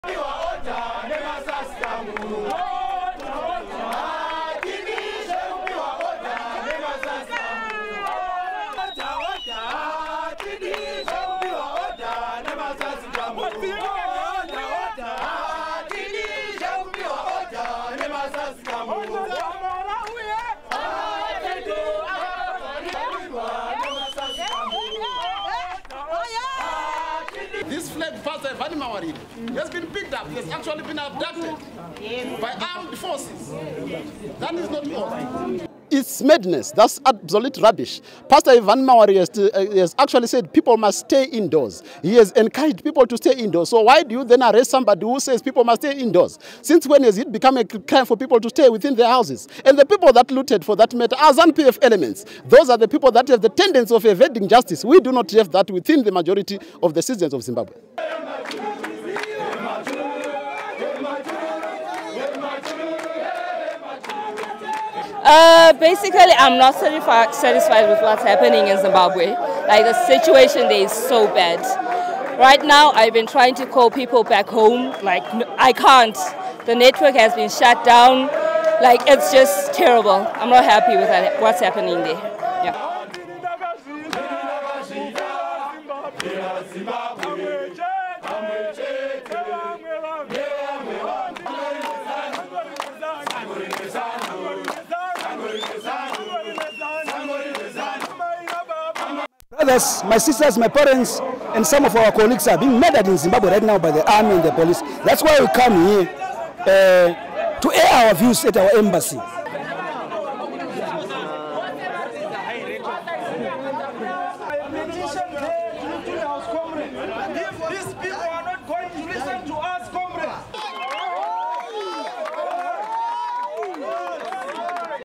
Zimbabweans protest outside embassy in Pretoria
(16 Jan 2019) Zimbabwe nationals living in South Africa gathered outside the Zimbabwe embassy to protest against a recent fuel hike and the shutdown of internet networks in their homeland.
Protesters sang and danced outside the embassy carrying placards calling on President Mnangagwa to resign.
There were widespread reports of violence as Zimbabwe faced a third day of protests over what has become the world's most expensive gasoline.
This is the country's worst unrest since deadly post-election violence in August that saw six people killed.
Zimbabwe's largest telecom company, Econet, sent text messages to customers saying it had been forced by the government to shut down the internet service.